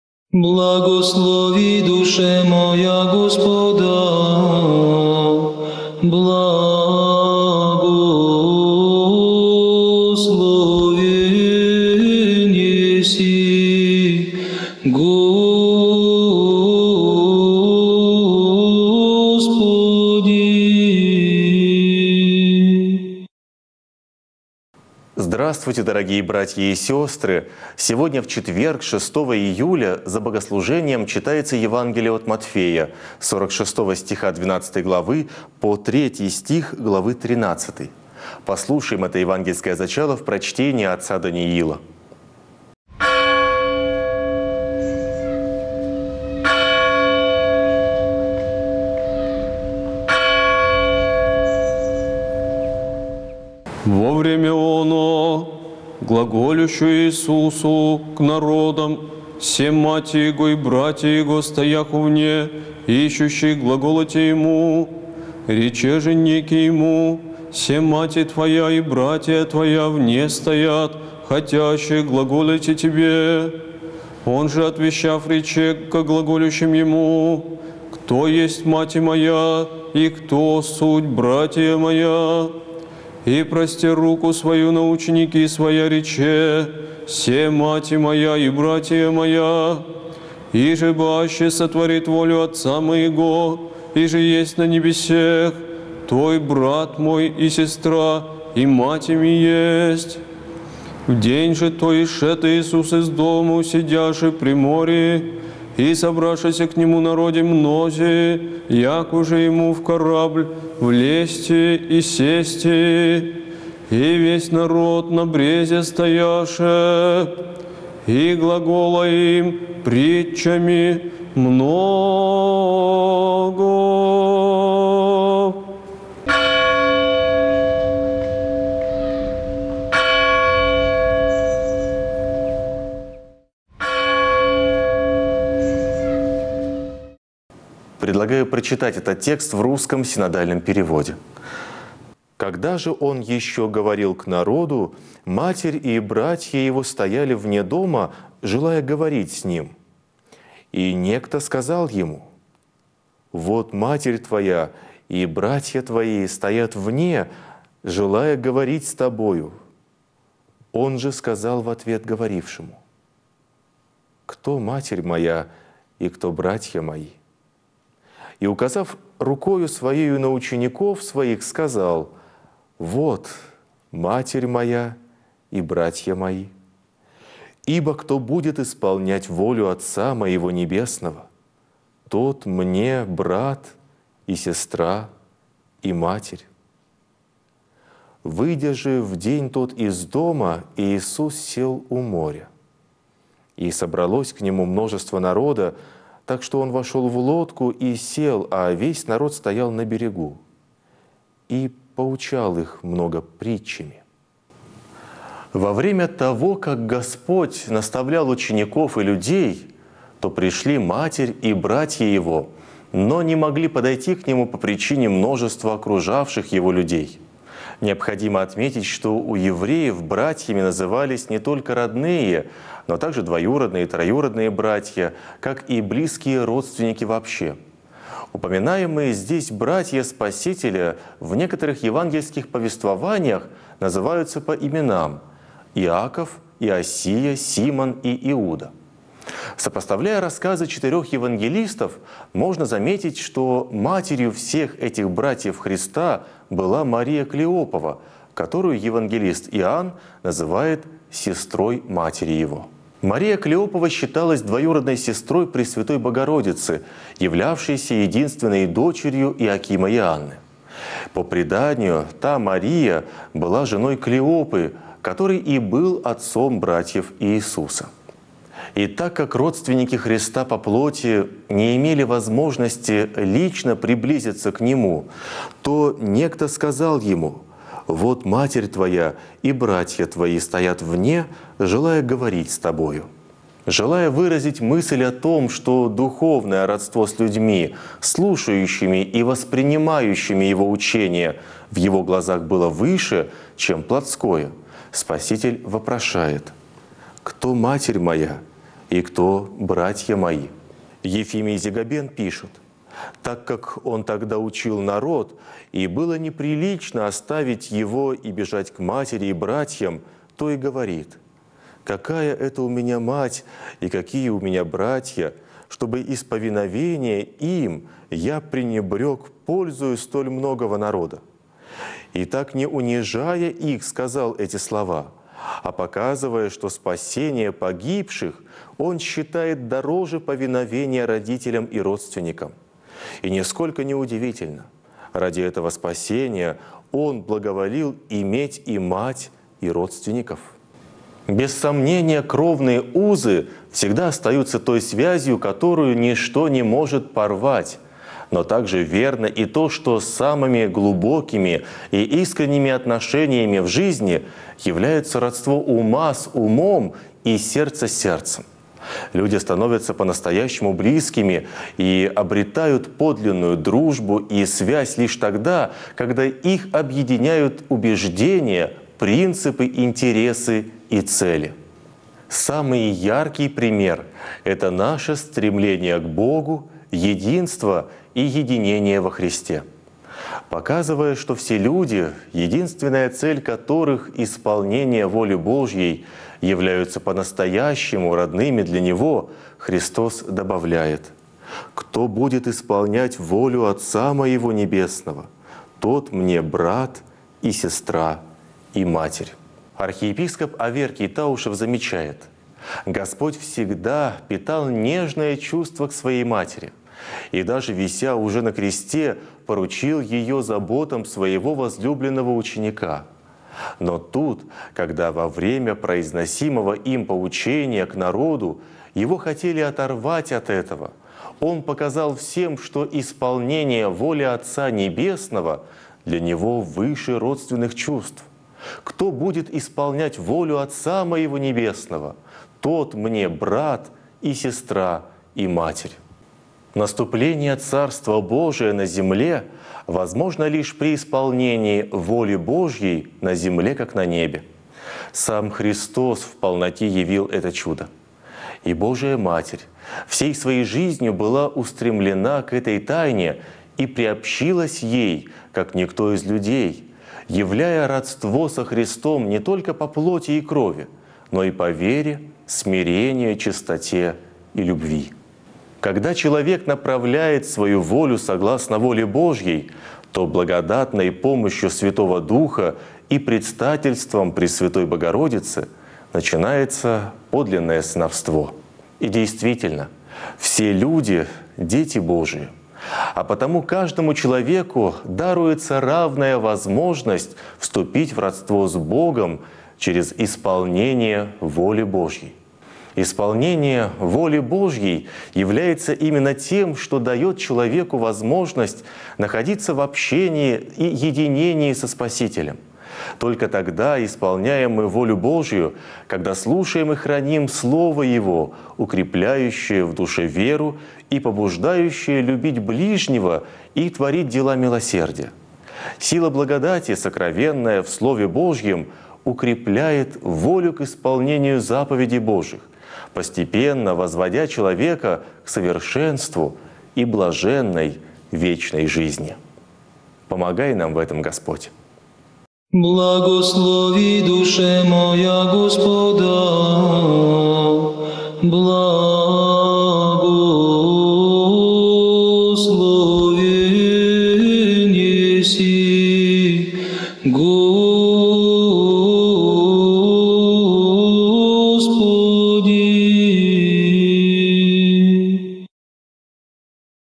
Читаем Евангелие вместе с Церковью. 6 июля 2017г
Святой Церковью читается Евангелие от Матфея. Глава 12, ст. 46–50; глава 13, ст. 1 – 3.
6 июля. Кто будет исполнять волю Отца Моего Небесного, тот Мне брат, и сестра, и матерь